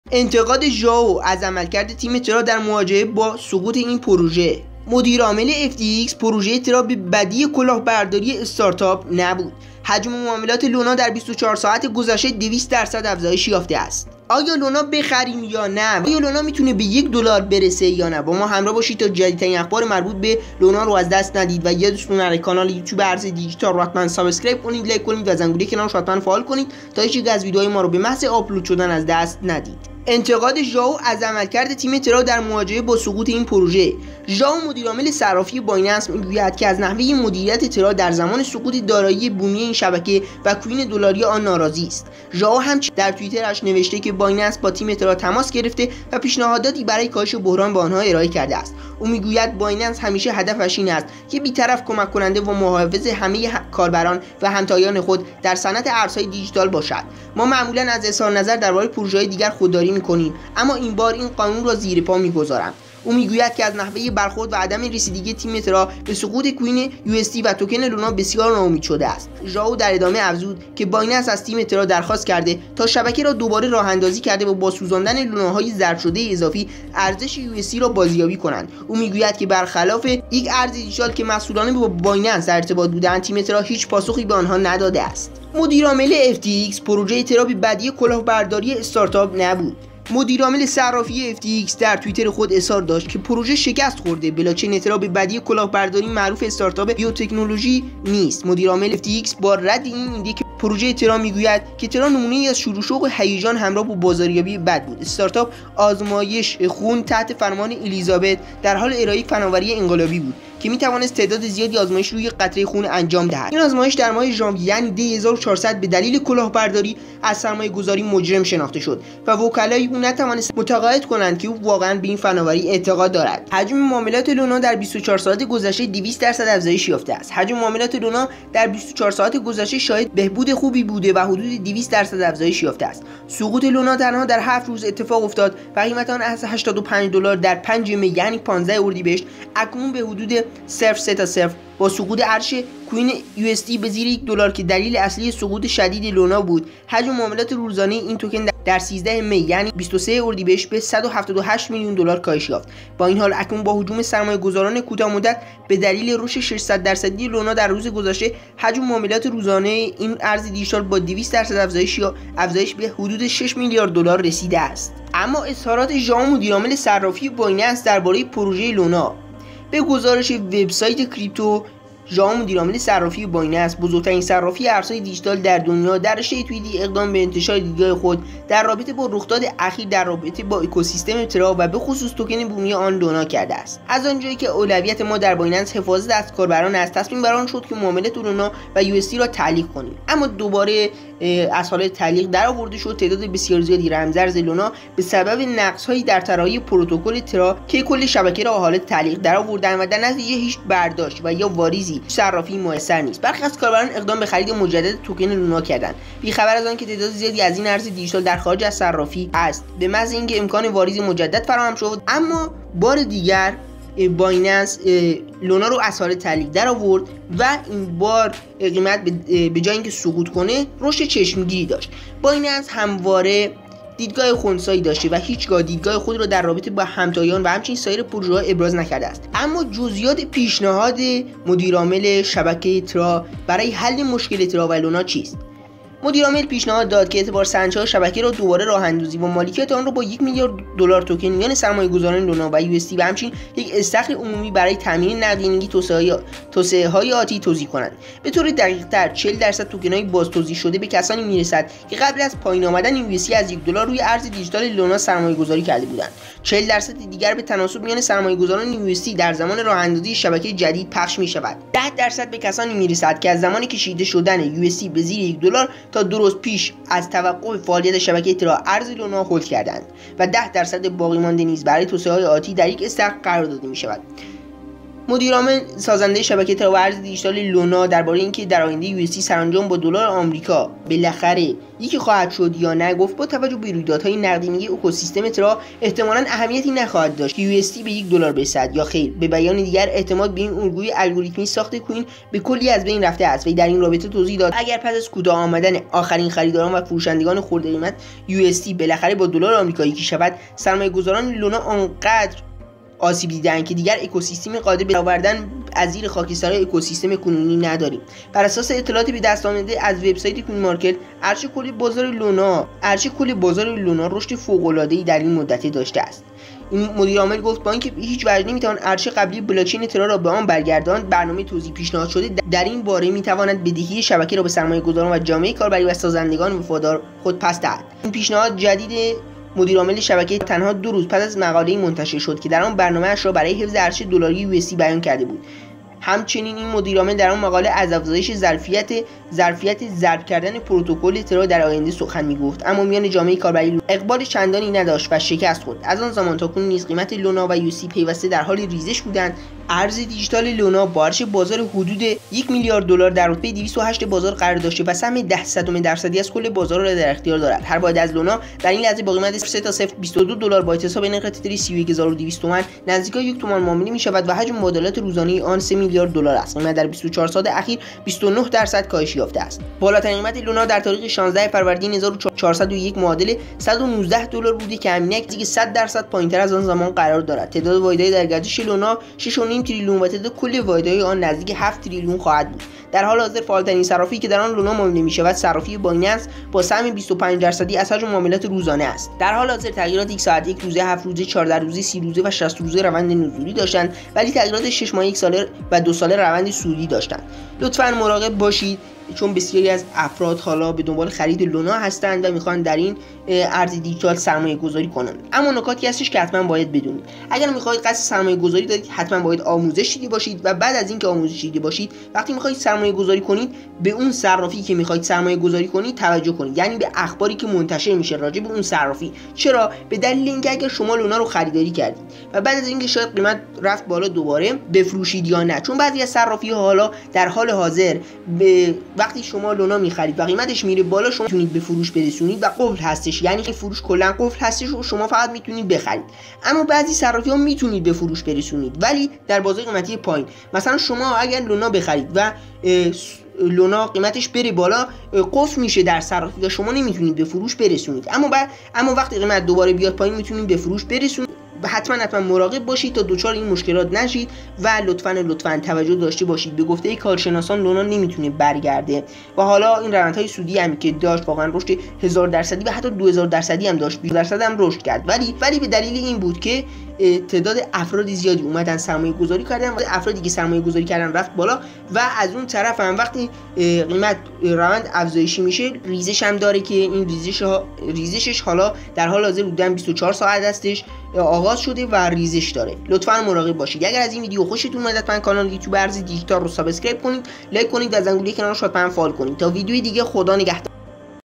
انتقاد ژائو مدیرعامل بایننس از عملکرد لونا؟حجم معاملات لونا ۲۰۰ درصد افزایش یافت؟لونا به۱دلار میرسد؟
سلام در این ویدیو به تحلیل و بررسی لونا پرداختم انتقاد ژائو مدیرعامل بایننس از عملکرد لونا؟ حجم معاملات لونا ۲۰۰ درصد افزایش یافت؟ لونا به ۱ دلار میرسد؟ با ما همراه باشید.